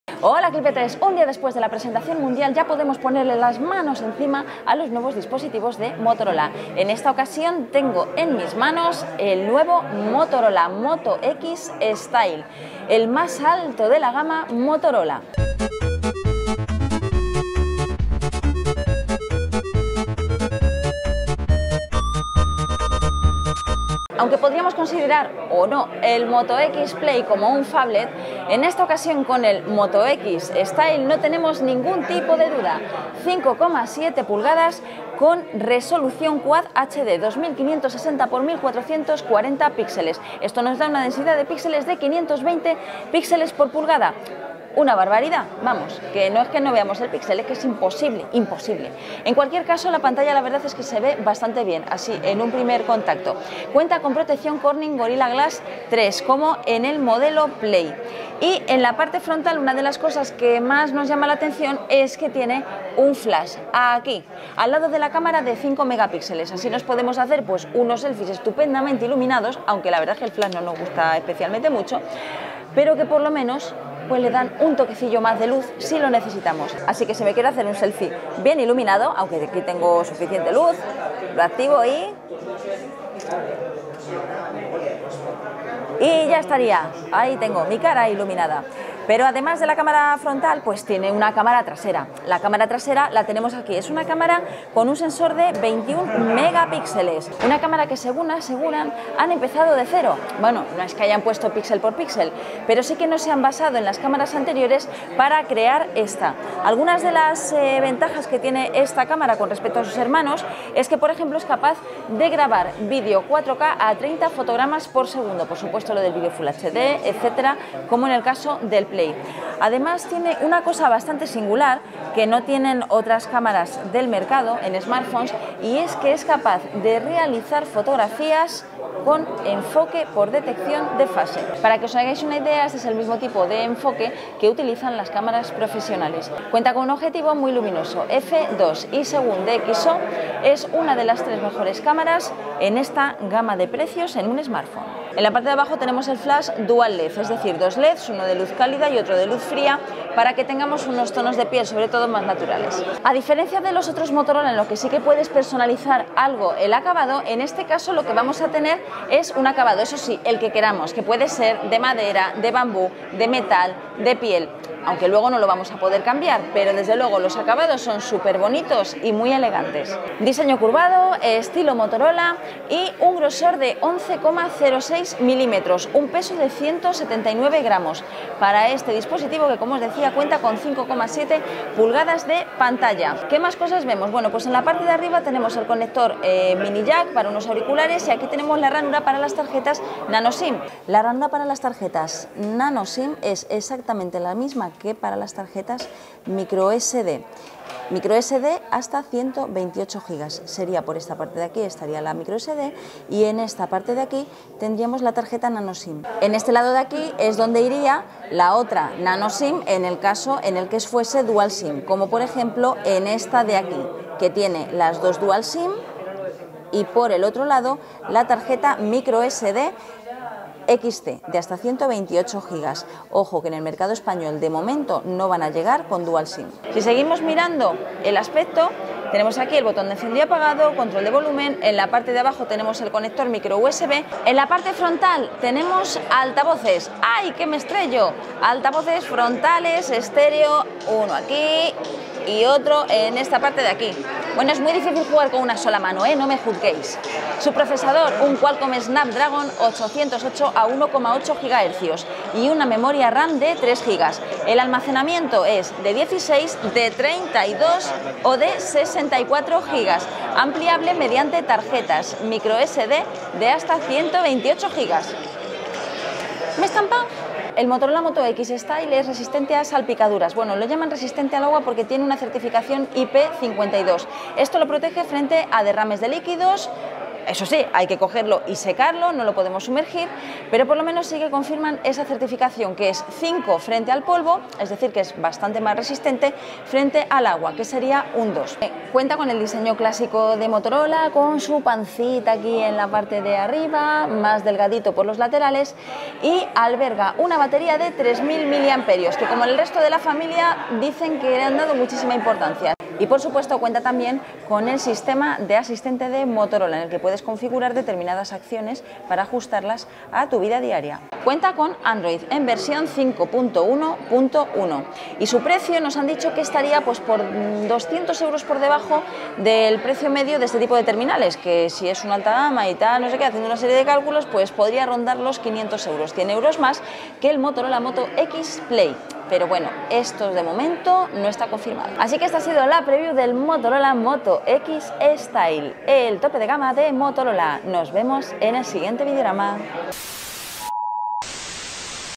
Motorola Moto X Style preview en español
rápidamente nos hemos ido a probar los nuevos móviles de Motorola y el primero que ha caído en nuestras zarpas ha sido el Moto X Style, el phablet con pantalla de 5,7 pulgadas y una gran inversión tecnológica en su cámara de fotos de 21 Mpx que asegura hacer unas fotos extraordinarias. de momento este es el primer contacto

+ info en clipset